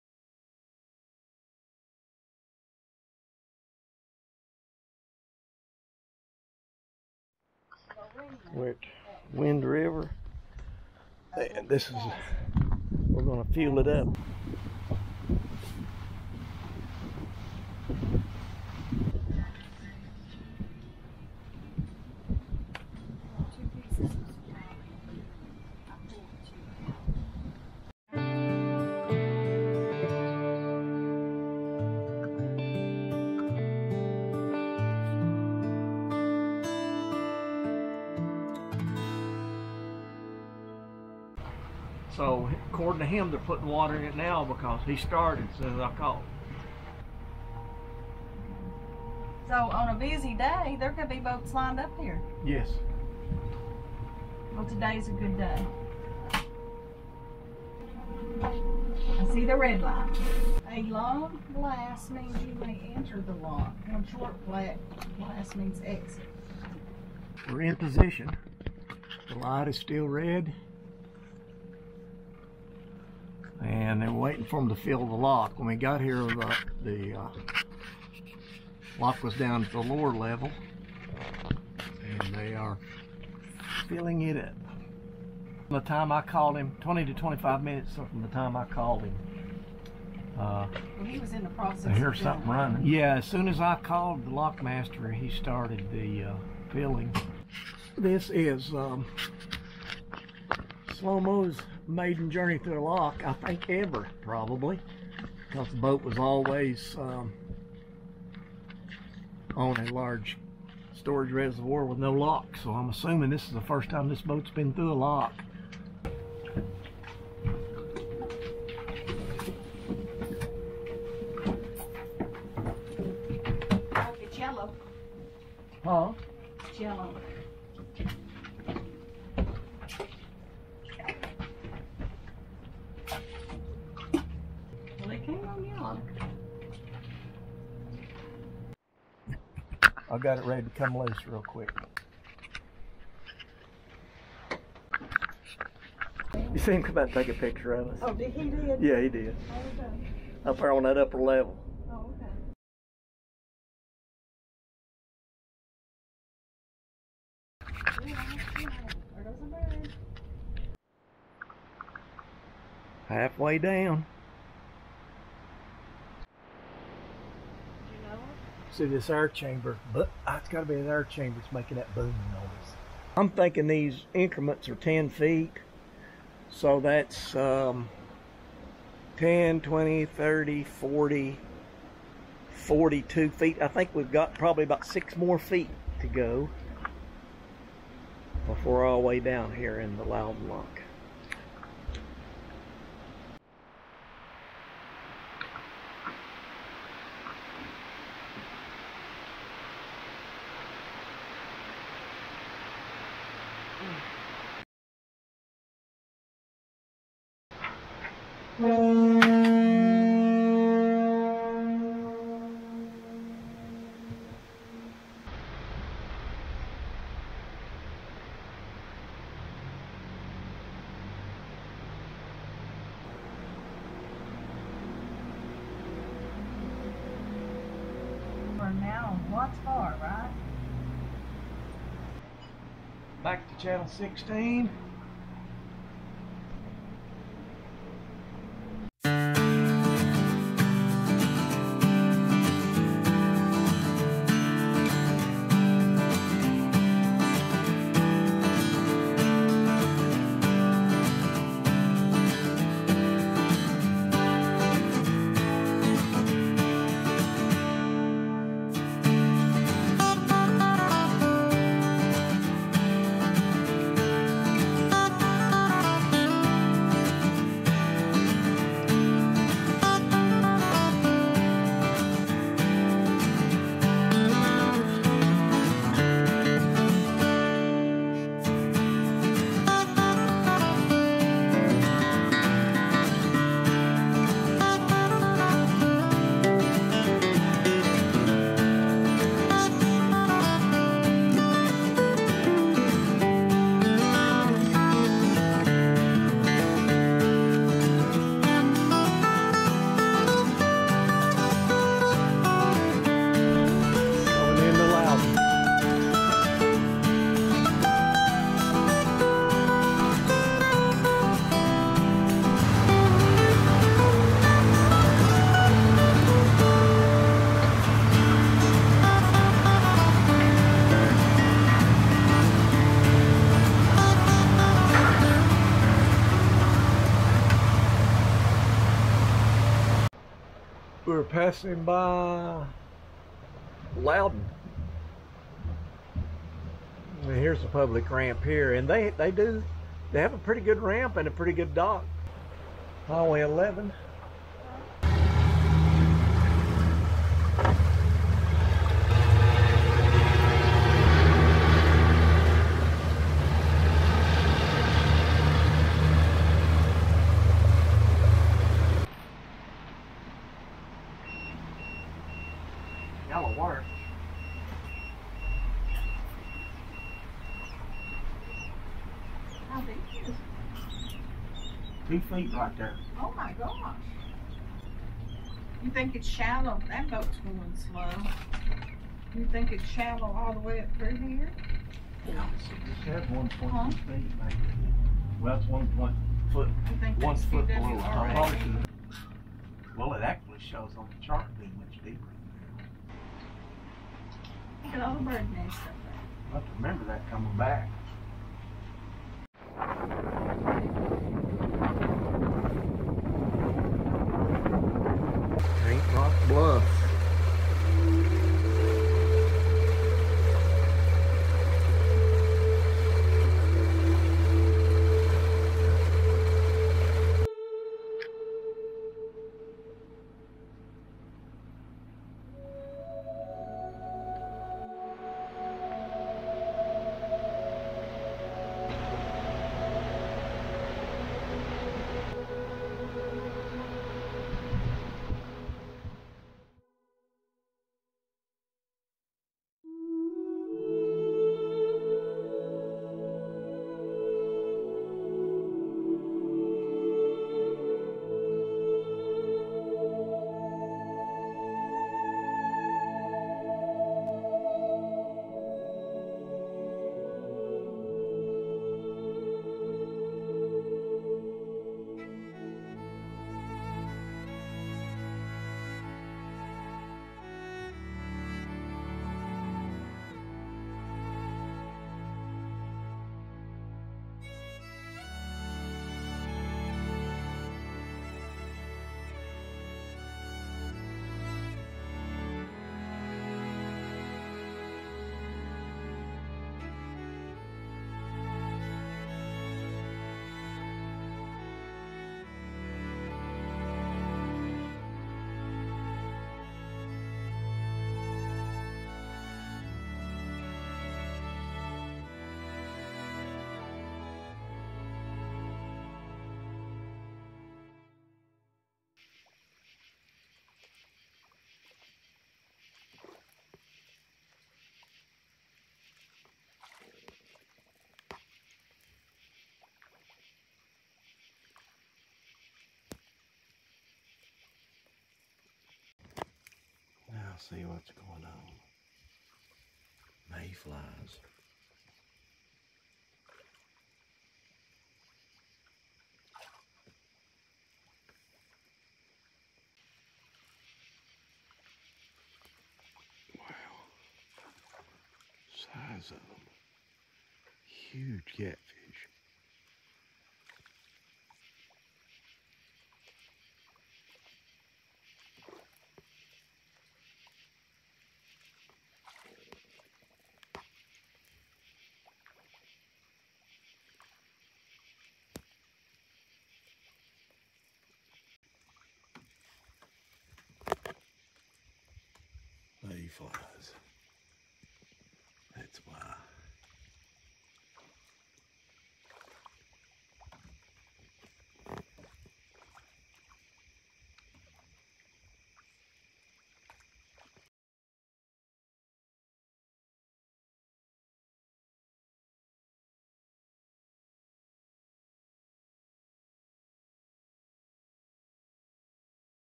(Part I) Catamaran Cruiser "SLO-MO" Houseboat. Shakedown cruise. Locking through.
This is the final trial before our Cumberland-Tennessee River Discovery trip, which begins soon.  This outing was a 48-hour two-night cruise down the river to determine if we've got everything prepared.
It also was our first experience through a lock with this boat. We learned quite a bit.